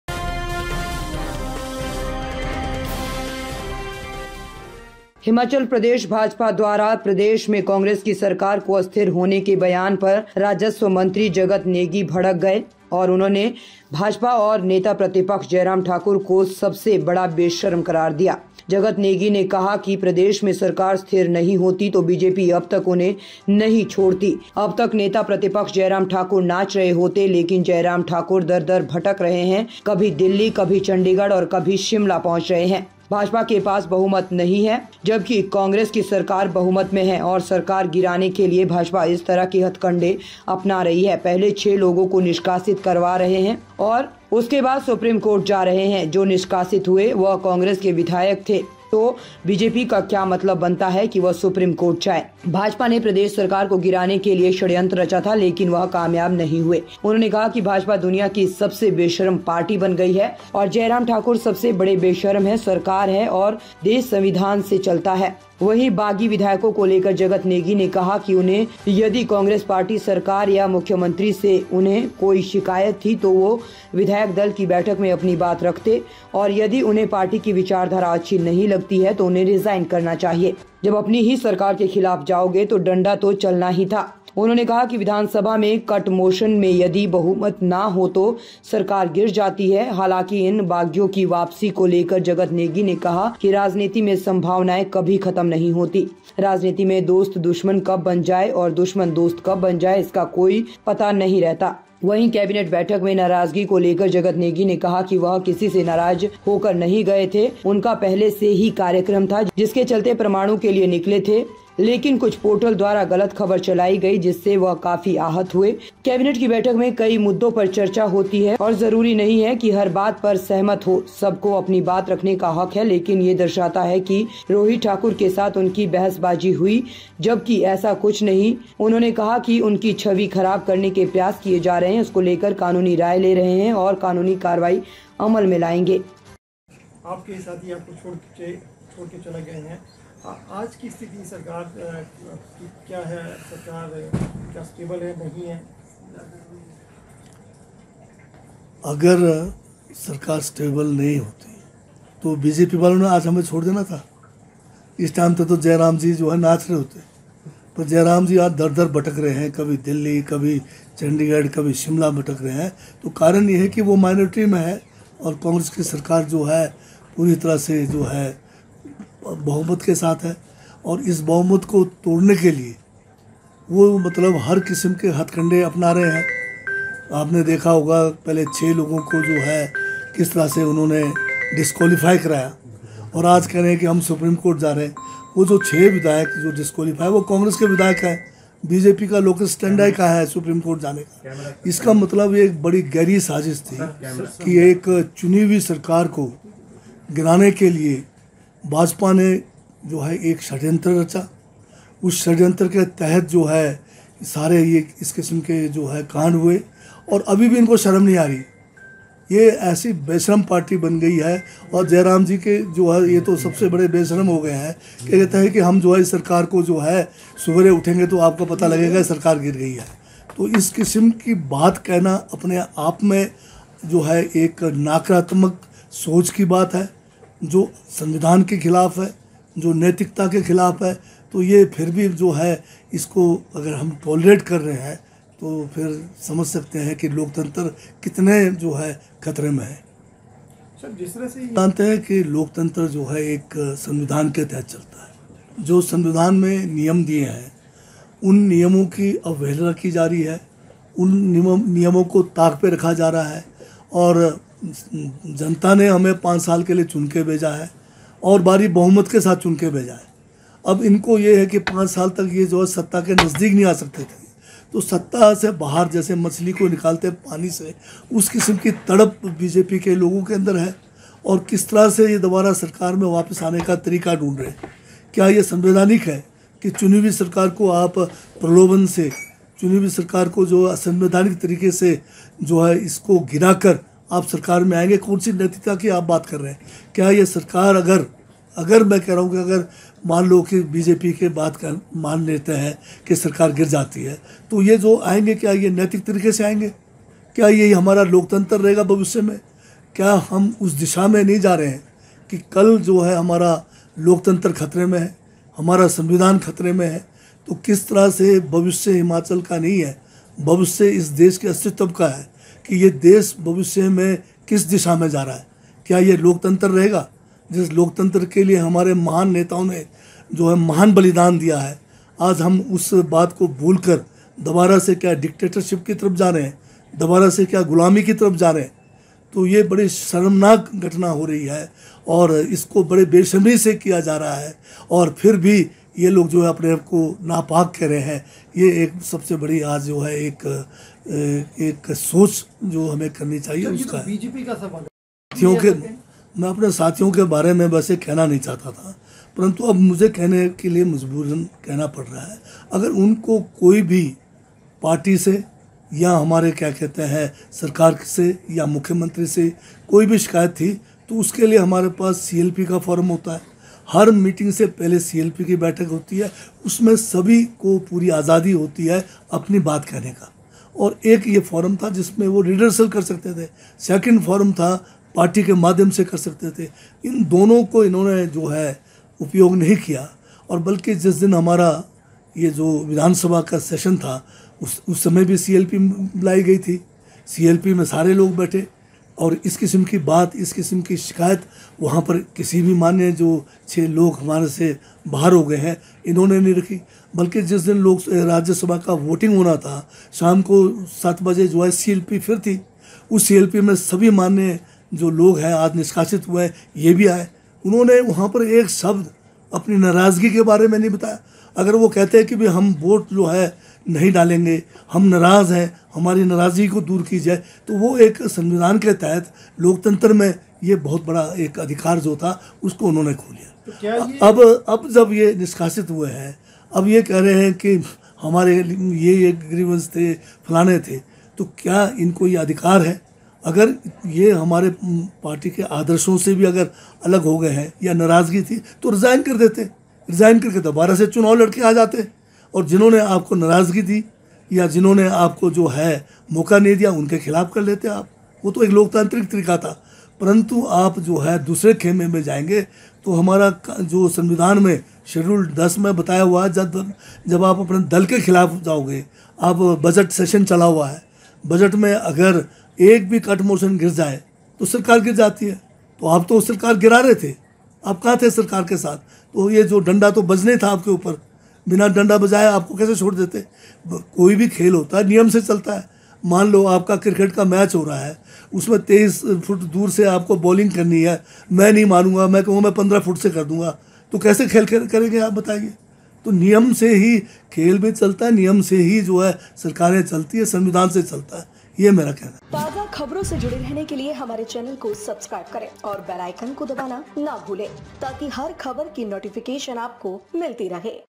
मंत्री जगत नेगी बोले सबसे बड़ी बेशर्म पार्टी है भाजपा, जयराम सबसे बड़े बेशर्म
मंत्री जगत नेगी बोले सबसे बड़ी बेशर्म पार्टी है भाजपा, जयराम सबसे बड़े बेशर्म,  सरकार अस्थिर न होती तो अब तक नाच रहे होते नेता प्रतिपक्ष जयराम ठाकुर, सरकार गिराने के लिए अपनाएं हथकंडे